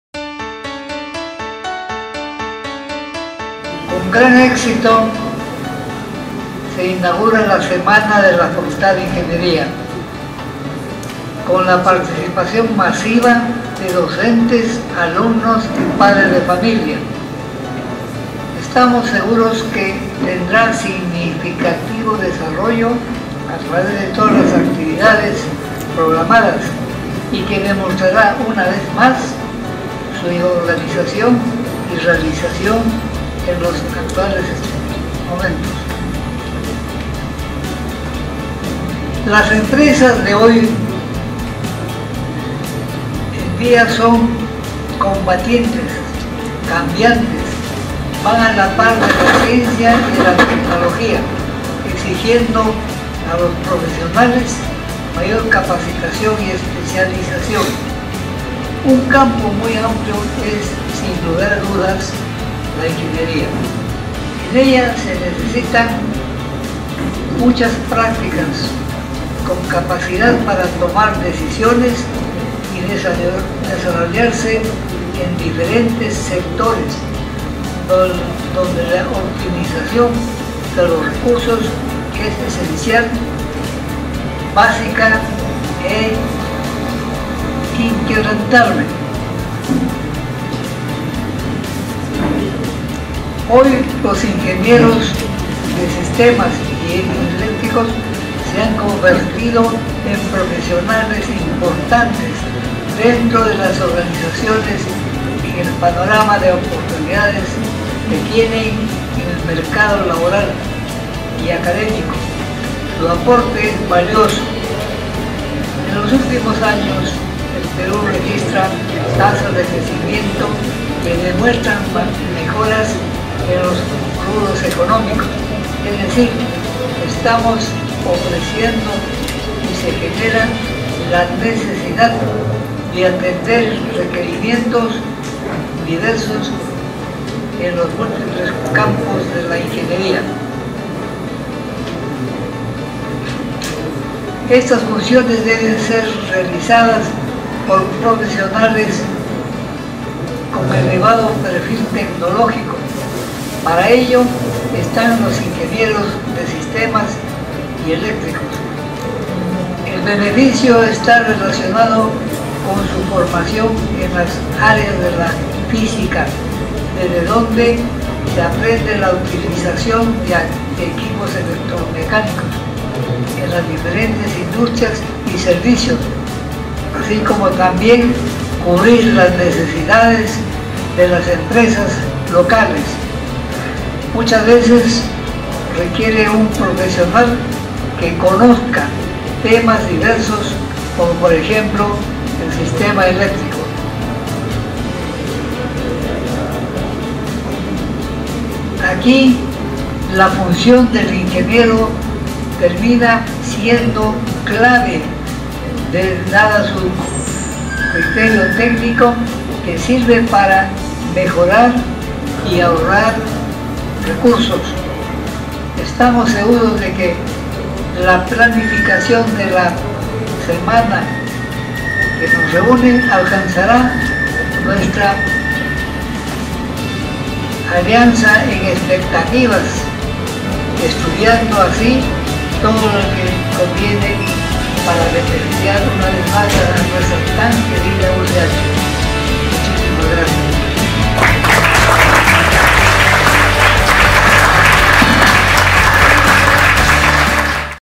Rector César Ángeles apertura Semana de la Ingeniería UCH
Dr. César Ángeles Caballero, Rector de la UCH inaugura la semana de Ingeniería en la Universidad de Ciencias y Humanidades (UCH) felicitando y motivando a los alumnos a convertirse en profesionales íntegros con una visión de responsabilidad social en beneficio de la comunidad.